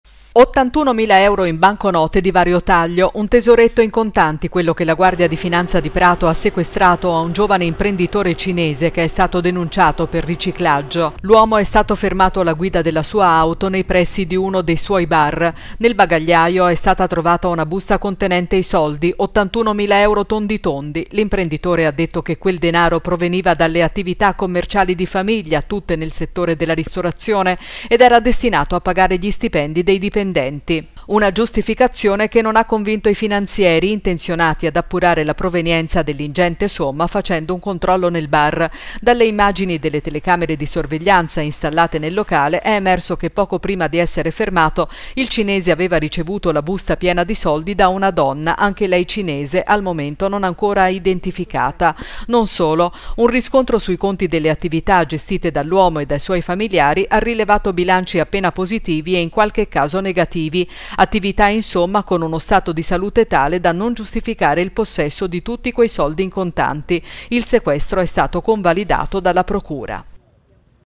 2022-01-19 PRATO - 81MILA EURO IN CONTANTI, DENUNCIATO IMPRENDITORE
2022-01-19 PRATO - 81MILA EURO IN CONTANTI, DENUNCIATO IMPRENDITORE